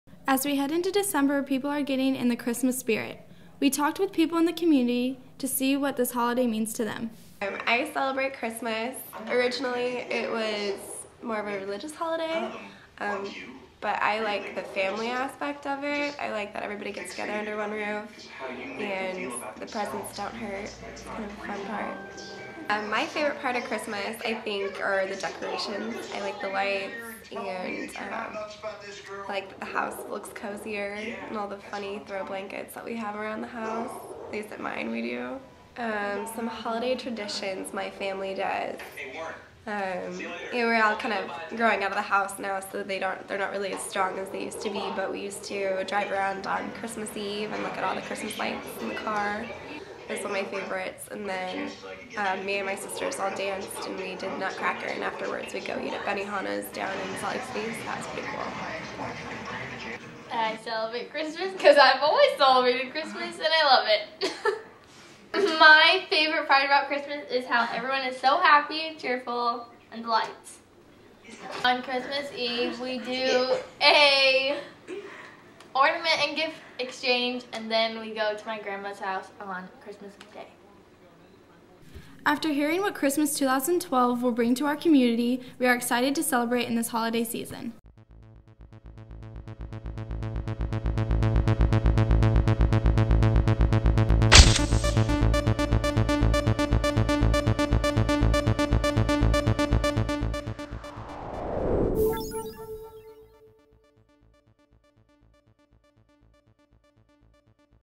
People in the Community are Getting in the Christmas Spirit.
Clackamas county residents tell us about their family traditions.